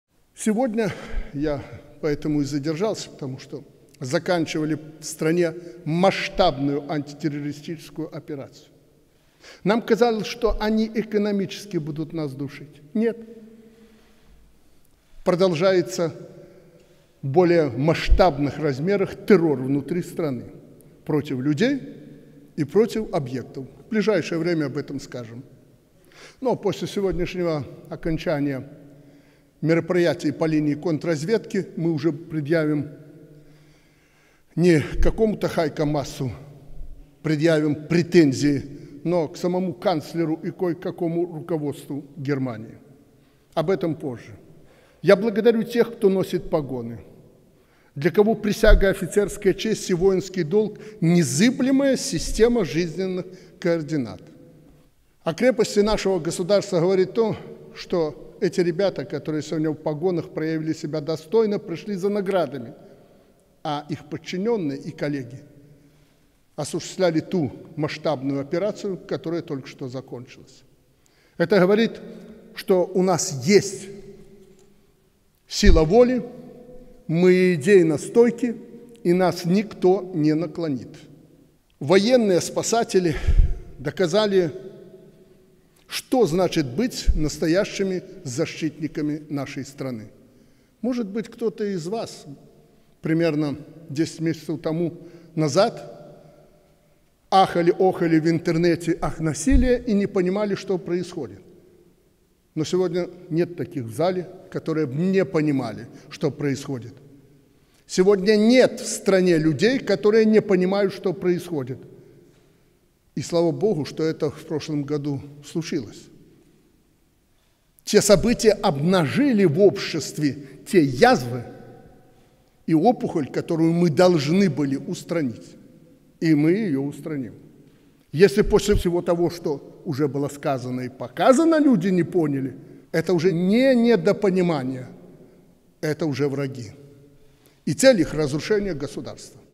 Лукашенко: Мы предъявим претензии самому канцлеру и кое-какому руководству Германии!
Лукашенко: Сегодня заканчивали в стране масштабную антитеррористическую операцию. Нам казалось, что они экономически будут нас душить. Нет. Продолжается в более масштабных размерах террор внутри страны против людей и объектов. В ближайшее время мы об этом скажем! После сегодняшнего окончания мероприятий по линии контрразведки мы уже не какому-то Хайко Маасу, министру иностранных дел Германии, предъявим претензии, но к самому канцлеру и кое-кому из руководства Германии. Об этом позже.

В Беларуси была проведена масштабная антитеррористическая операция. Об этом Президент страны Александр Лукашенко заявил 2 июля на церемонии вручения государственных наград во Дворце Независимости.